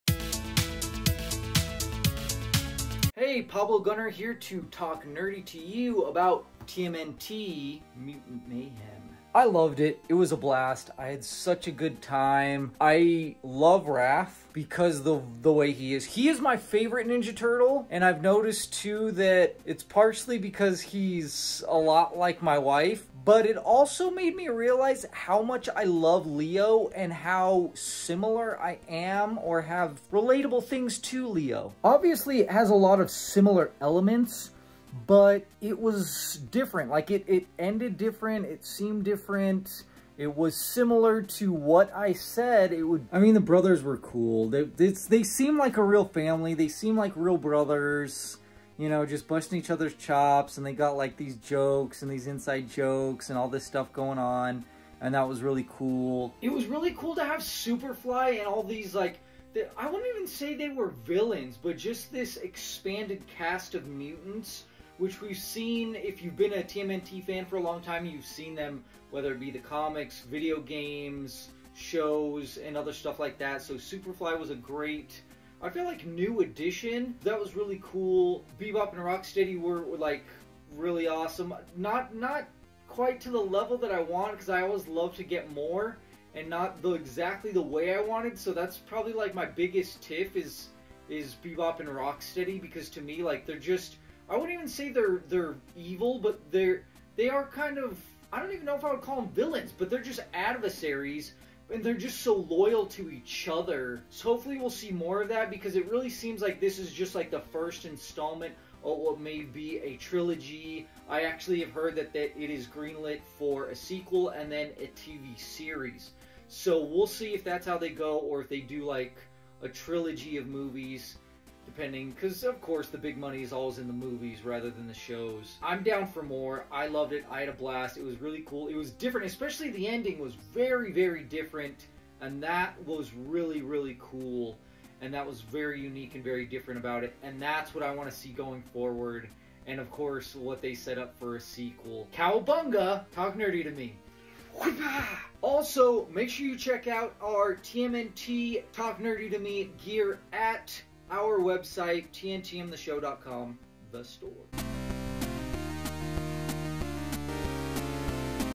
TMNT Mutant Mayhem Spoiler Free TALK NERDY TO ME
Pablo Gunner gives you the spoiler free down low on Teenage Mutant Ninja Turtles as a life long fan of the TMNT cartoons, movies, and comics.